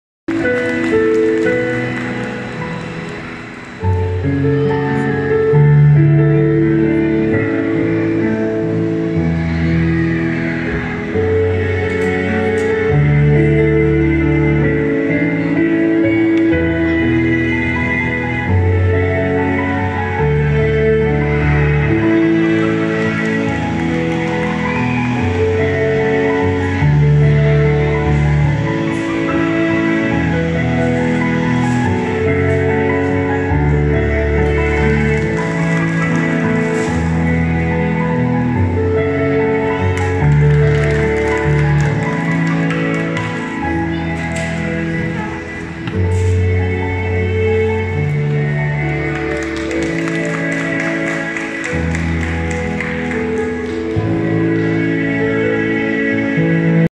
SHOW 4 MAGOS POKER DE ASES DEL PROGRAMA "MÉXICO TIENE TALENTO" PLAZA COACALCO. WHATS 551353-8159(4)
SHOW 4 MAGOS POKER DE ASES DEL PROGRAMA "MÉXICO TIENE TALENTO" PLAZA COACALCO.